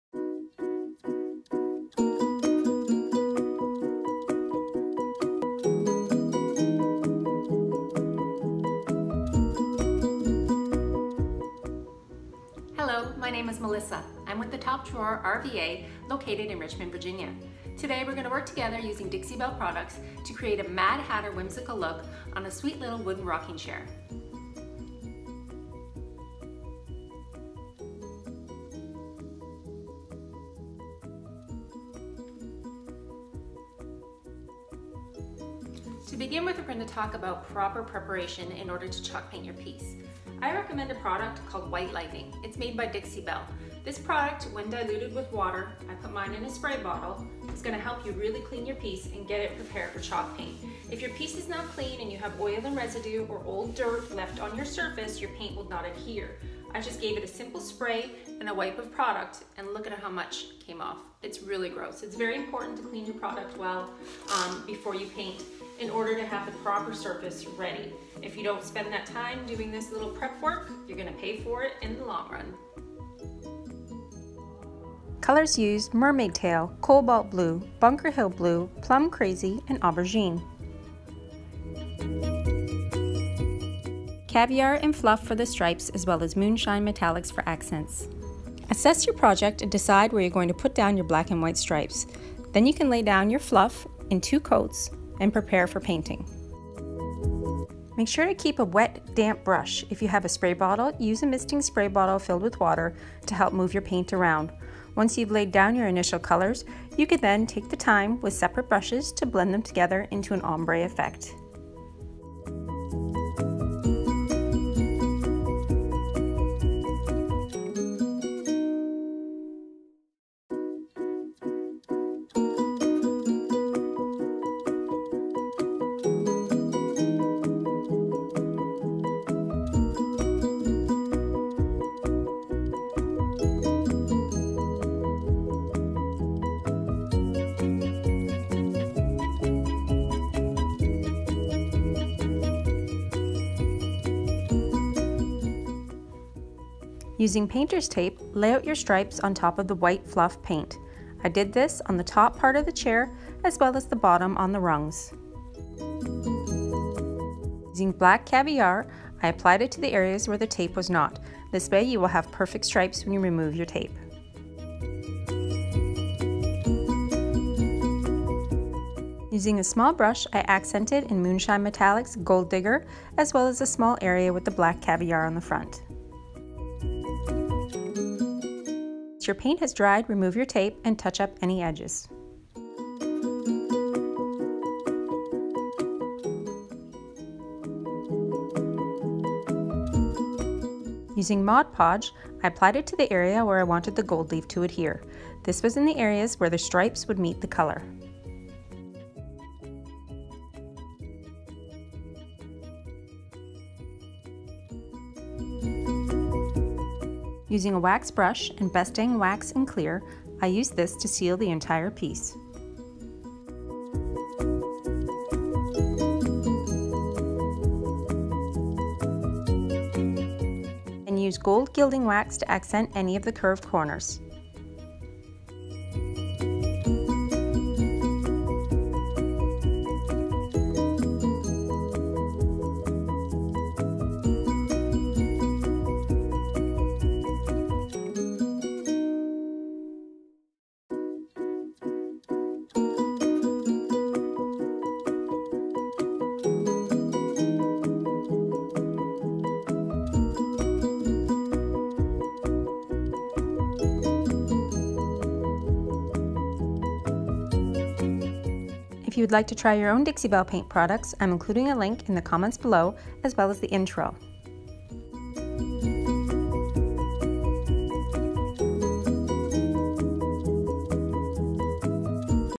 Mad Hatter Rocking Chair Paint Makeover - How To Paint Whimsy & Stripes
Stripes and whimsay on a fin ricking chair. Quick how to video on how to use Dixie Belle Paint products tp make over a wood chair!

Follow along on all social media @thetopdrawerrva